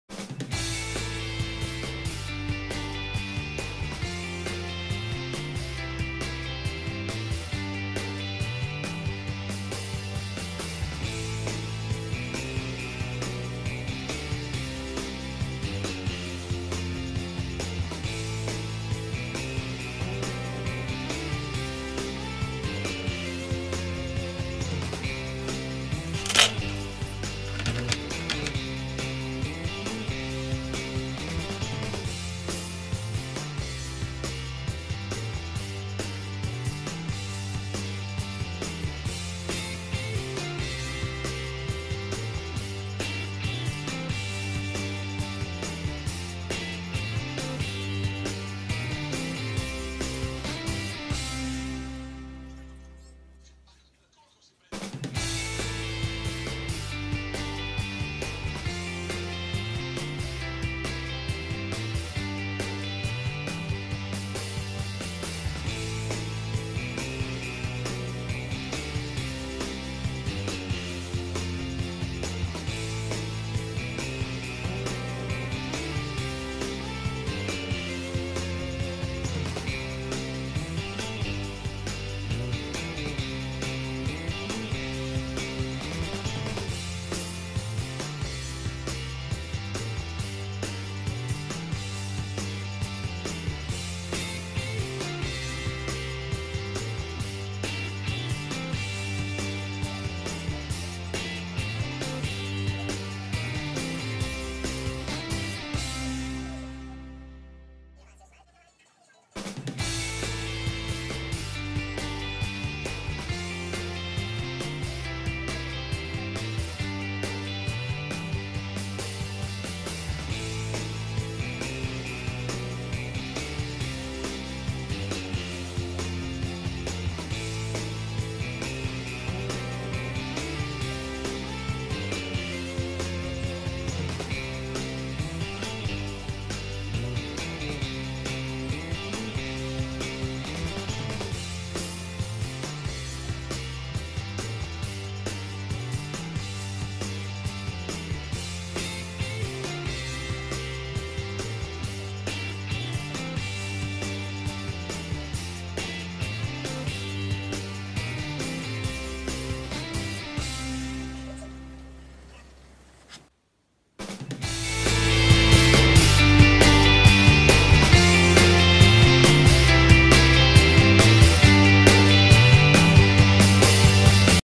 Realistic cactus - ART tutorial
Speed art tutorial
Realistic cactus
Watercolours , black & white markers
ArtTrochuInak -instagram